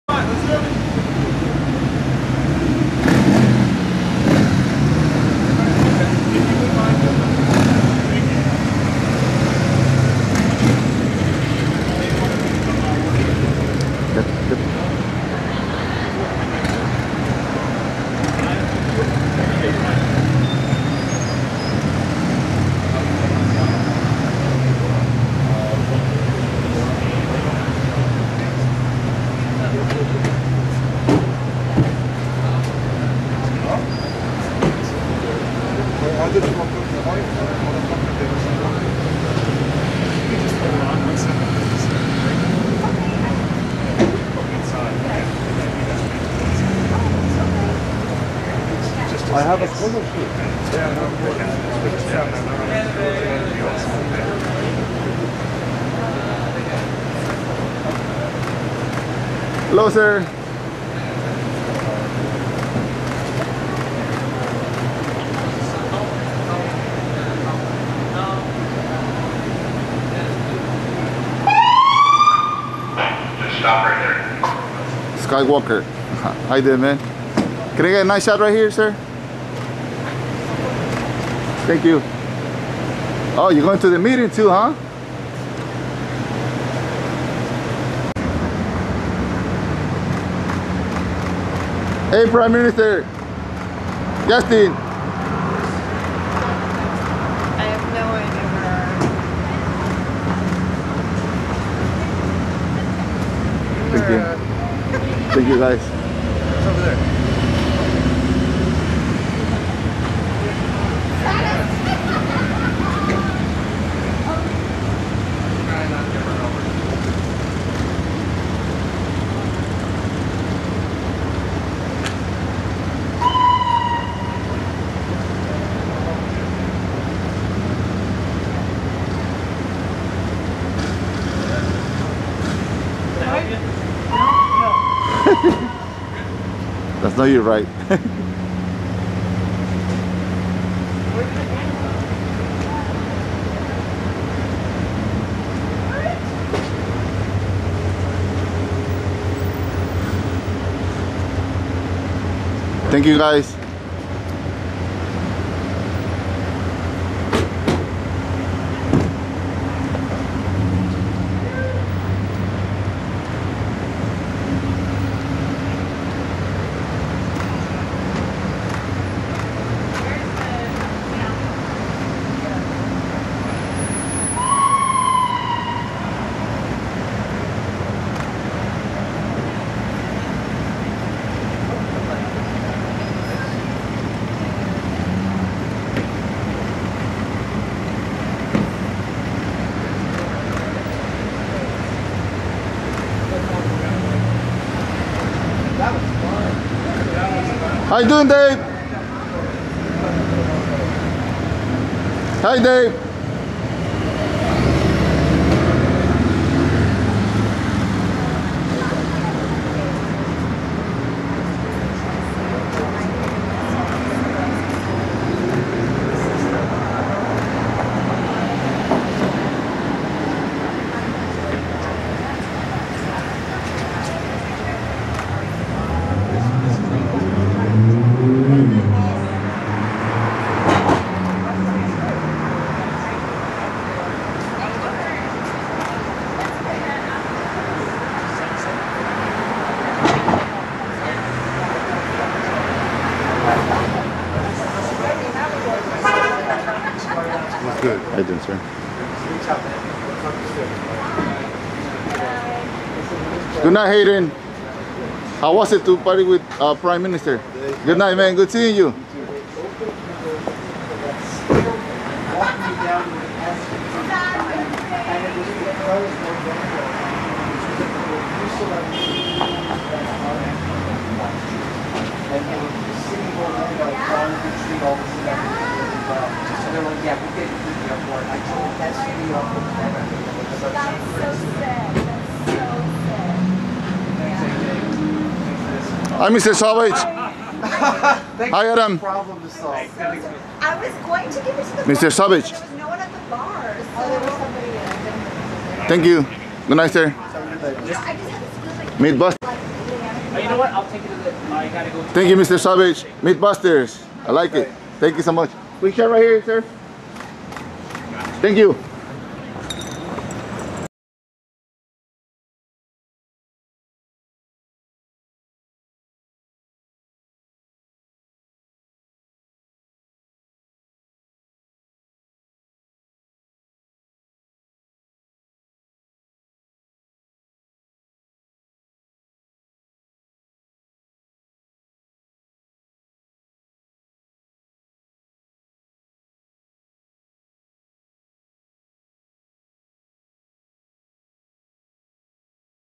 Justin Trudeau tests positive for COVID after meeting with Hayden Christensen at LAVO Ristorante
Canadian Prime Minister Justin Trudeau tests positive for COVID two days after meeting with Hayden Christensen at LAVO Ristorante in West Hollywood,CA
Video credit HEDO/BACKGRID ©️ Copyright